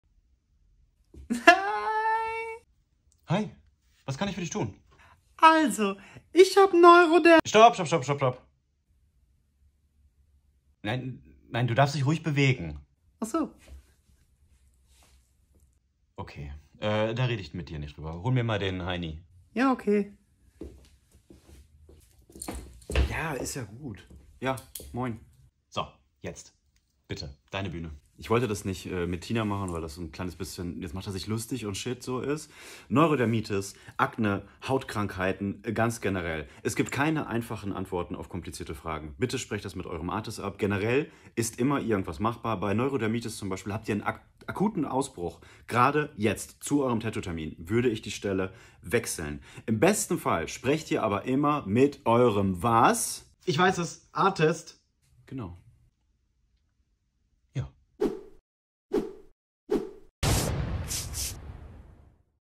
Tattoo Kunden: Hautkrankheiten
Nachtrag:
Logischerweise sprecht ihr bitte zuerst mit eurem ARZT (!), was ich jetzt einfach mal vorausgesetzt habe 😄
Shirts, Tassen und Merch?